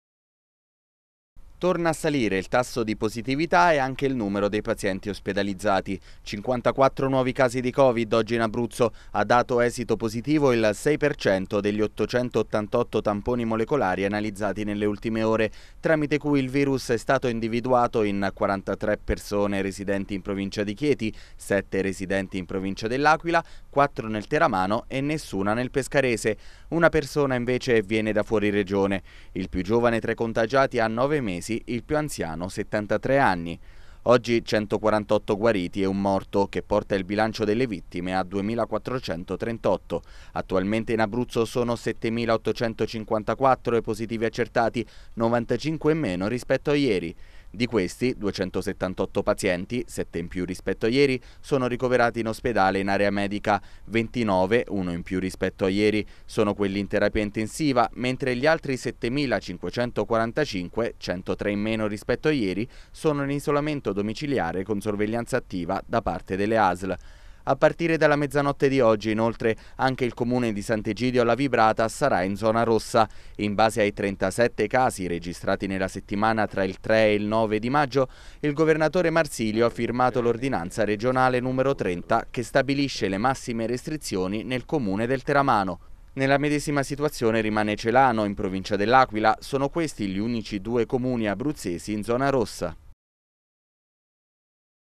Covid Abruzzo. 54 nuovi casi, Sant'Egidio alla Vibrata diventa zona rossa
Bollettino Covid: oggi 54 nuovi casi in Abruzzo (6%), 148 guariti e un morto. Torna a salire il numero degli ospedalizzati e, per effetto di ordinanza regionale, dalla mezzanotte di oggi passerà in zona rossa anche Sant'Egidio alla Vibrata.